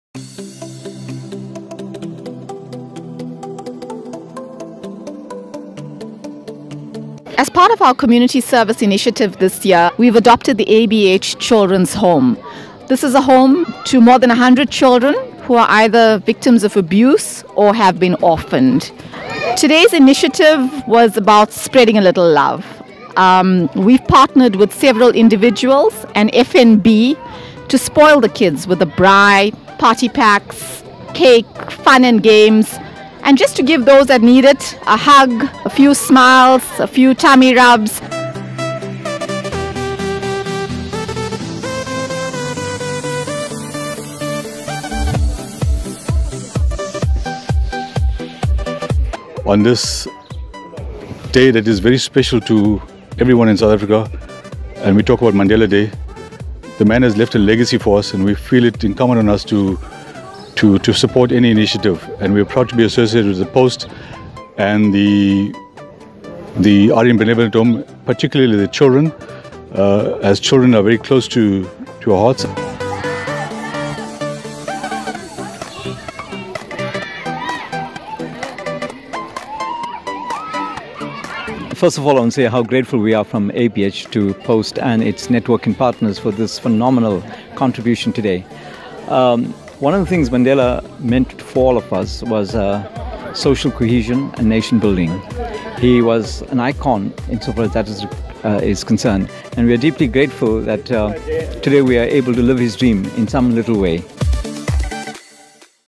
The POST Newspaper gives back for Mandela Day 2016
The children of the Aryan Benevolent Home (ABH) were treated to a fun-filled day of face painting, clowns, pony rides and a scrumptious braai for Mandela Day.